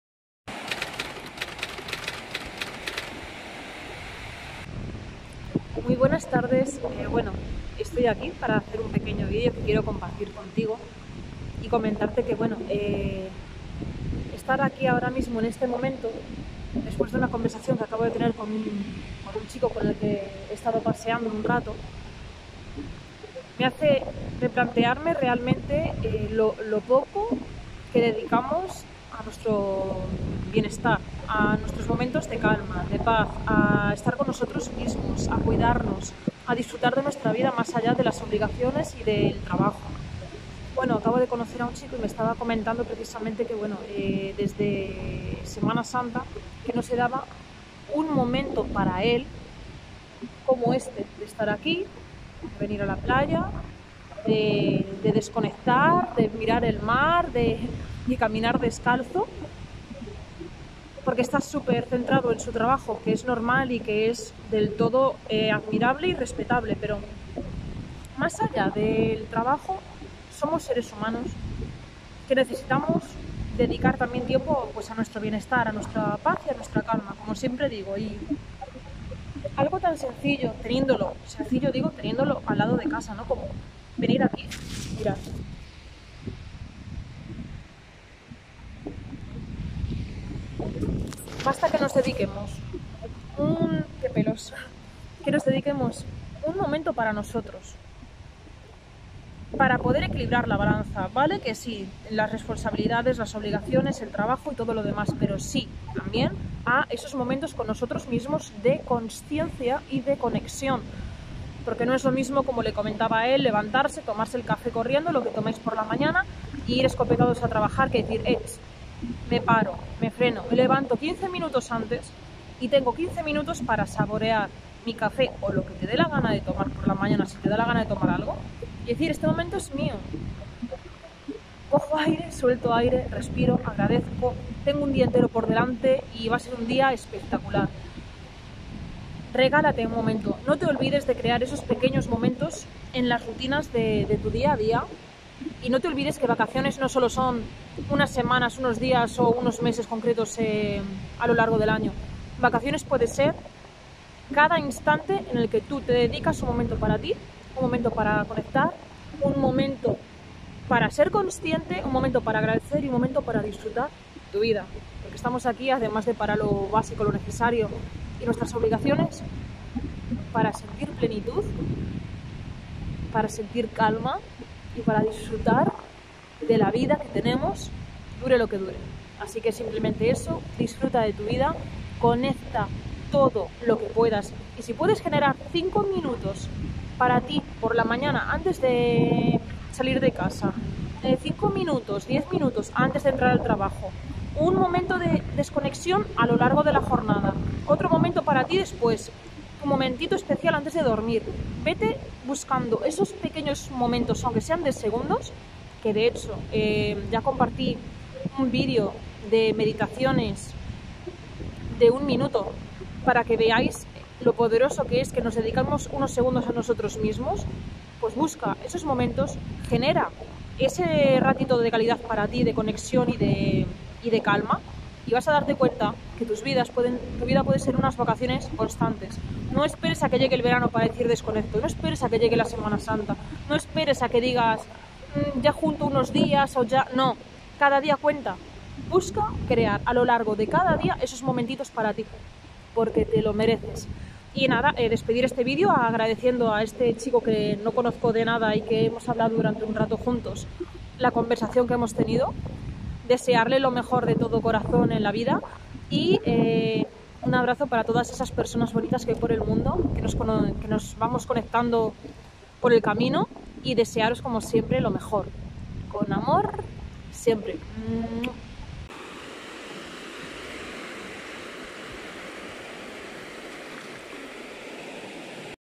VIVIR ES URGENTE! 🏖 ❤️No esperes al Verano para estar de VACACIONES ☀
No esperes al verano para estar de vacaciones. La vida es AHORA. 🏖 ❤️
Quería compartir contigo una pequeña reflexión... No esperes, VIVIR ES URGENTE!

Te mando AMOR ❤️🌹 🍀